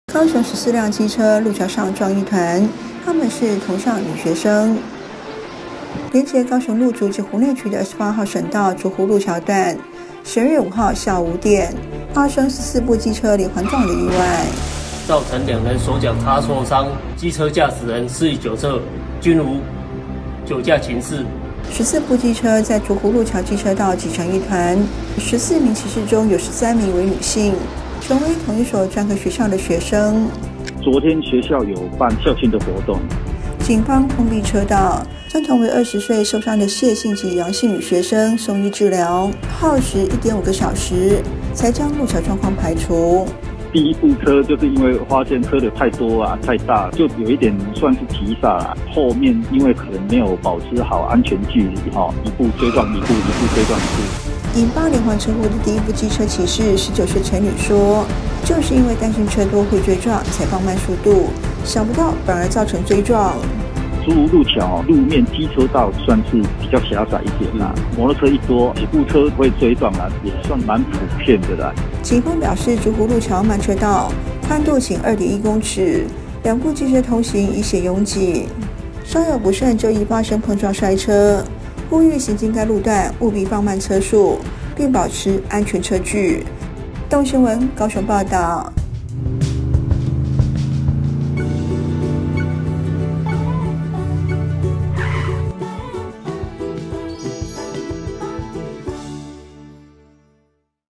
高雄14輛機車陸橋上撞一團 她們是同校女學生 | 台灣 蘋果新聞網
高市路竹區1處陸橋發生14部機車連環撞，造成2名學生受傷，所幸僅是手腳的擦挫傷，包紮後無大礙。警方指出，發生車禍都是來自同所專科學生，14人中13人為女性，僅1名男性。警方除了將受傷學生送醫外，還就近借用學校場地問製12名學生筆錄，初步判定未保持安全車距釀禍。